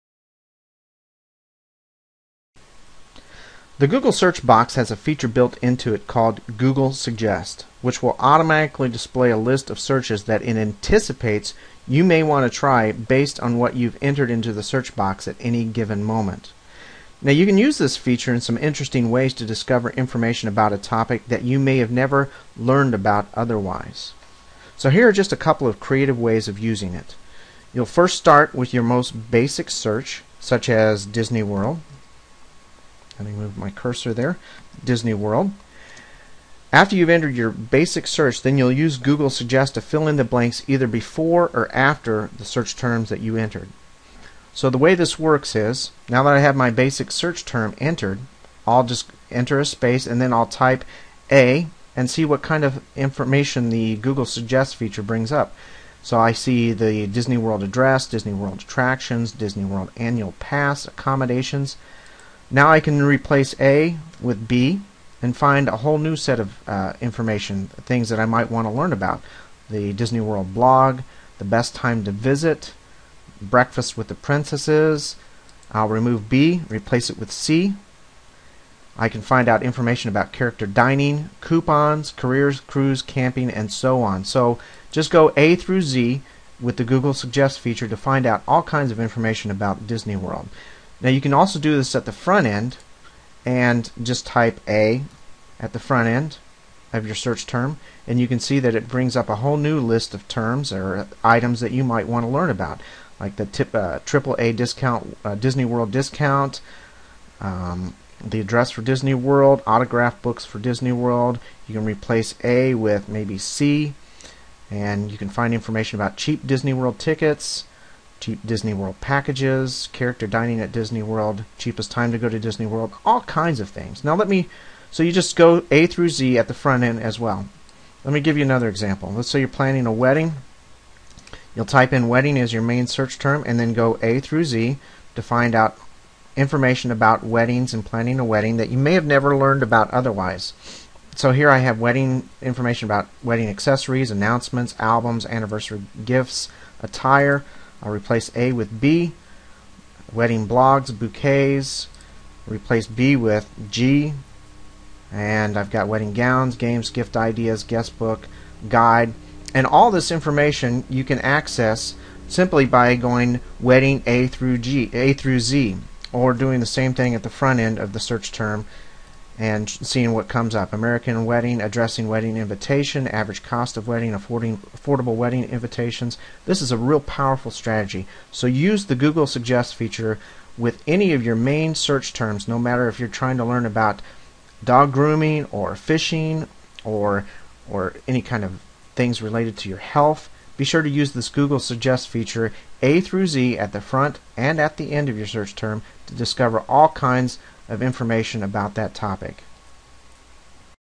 Explore A Topic Completely Using Google Suggest
The Google search box has a feature built into it called Google Suggest, which will automatically display a list of searches that it anticipates you may want to try, based on what you've entered into the search box at any given moment.
You can use this feature in interesting ways to discover information about a topic you may have never learned about otherwise.
Here are just a couple creative ways of using it way.
Start with the most basic search, such as disney world, then use Google Suggest to fill in the blanks, either before, or after, the search terms you enter:
disney world [a-z]
[a-z] disney world
Here's another example:
wedding [a-z]
[a-z] wedding
Each suggestion is like an answer to a question you have about the topic.